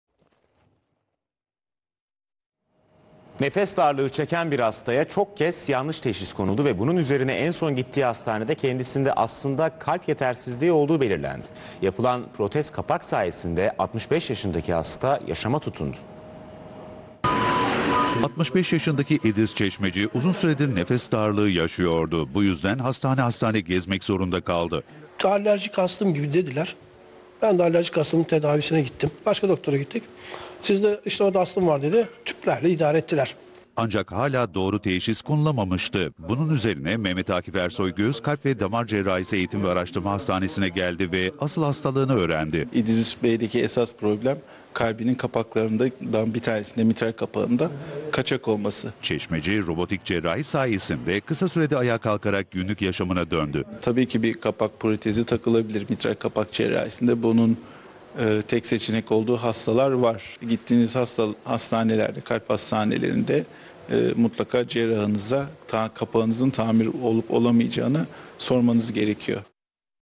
Mehmet Akif Ersoy GKDC EAH/ Tamir Edilen Kalp Kapakçığı ile Yaşama Tutundu/ Prof.Dr.Burak ONAN
Nefes darlığı nedeniyle birçok hastaneye başvuran ve mitral kapak yetersizliği olduğu öğrenen 65 yaşındaki İdris Çeşmeci, "Belki de orada ölebilirdim. Böyle bir ameliyat olduğum için yaşıyorum, ikinci bir yaşam. Herkes korkmadan gelsin, rahatlıkla kendilerini teslim etsinler" dedi.
hastamıza yönelik yapılan işlem hastanemiz Kalp ve Damar Cerrahisi uzmanlarından Prof.Dr.Burak Onan ve ekibi  tarafından Robotik Cerrahi yöntemi ile gerçekleştirildi.